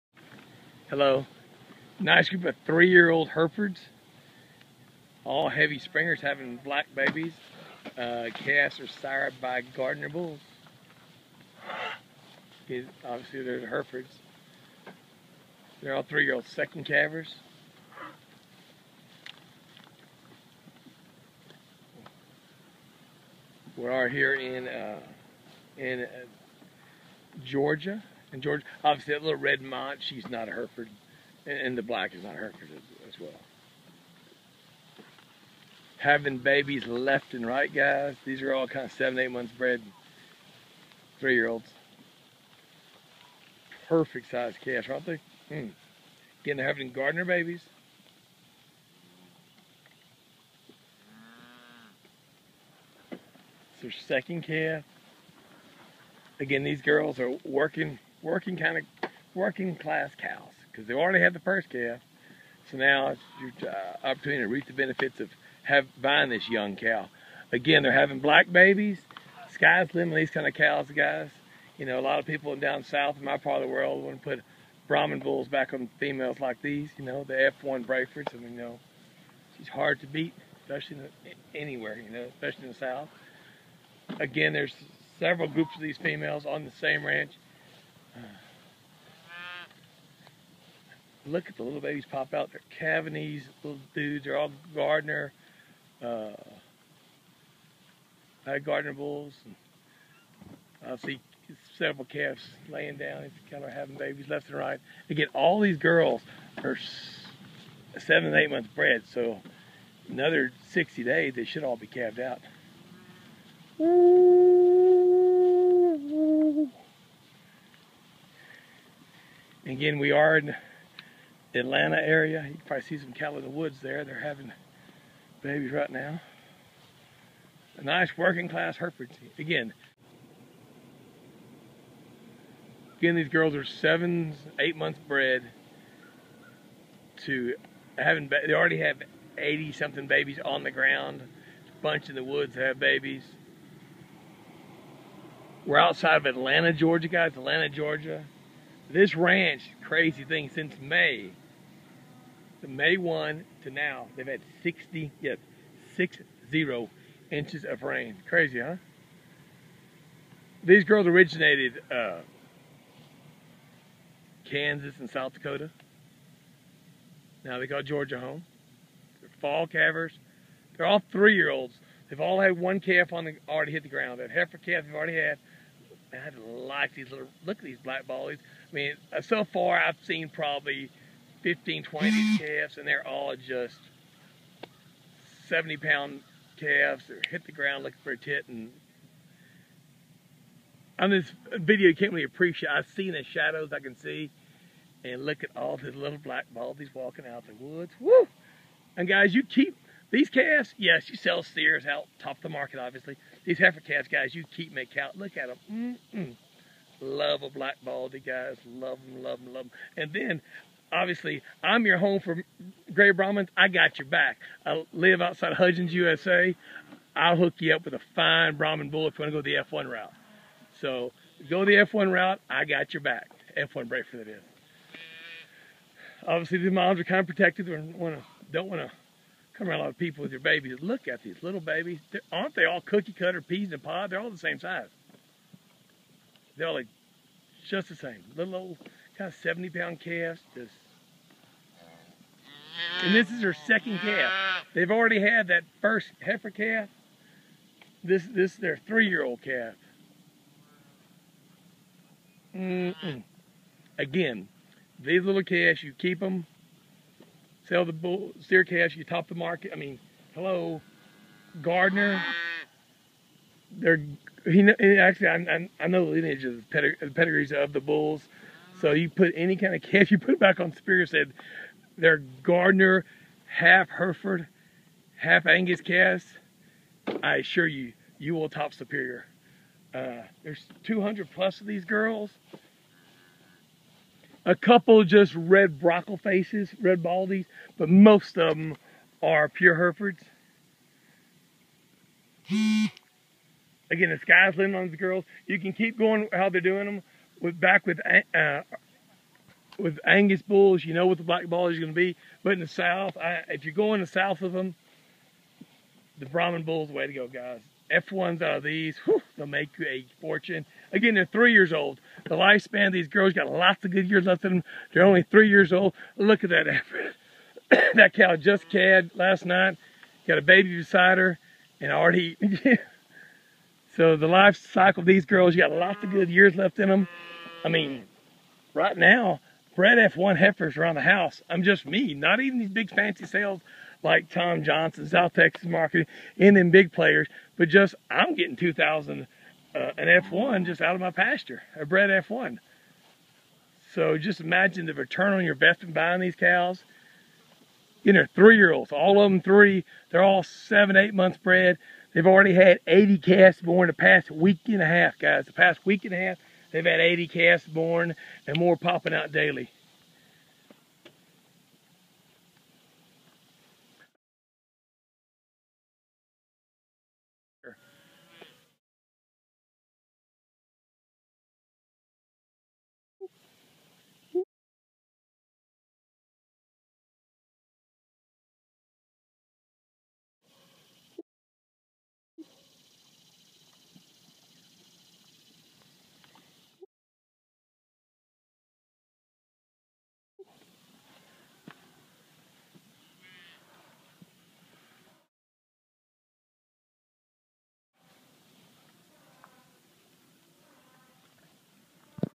Ankony Herefords
Here is a young, fancy,  group of 3yr. old Herefords Heavy bred to sons Gardiner  Predestine Ranch Angus Bulls with low birth and high growth numbers.   These numbers would place them in the top 2% of the population of the entire Angus breed.
You will love the Black Baldies these ladies will produce, the steer calves will command TOP Dollar on the feeder calf market and under proper management  the heifers could bring as much as the entire cows sell for!
Just think about the breeding opportunities these ladies bring to the table!!! In my opinion these ladies make tons of MONEY producing F1 Brafords Babies.  Keep in mind F1 Braford Producers these ladies are having babies NOW in just a few short months you can have your Brahman Bull covering these ladies with the fine black baldie babies!! These ladies are Truly a Center piece of any progressive stock seed stock producer business.
Enjoy the cattle,
Clay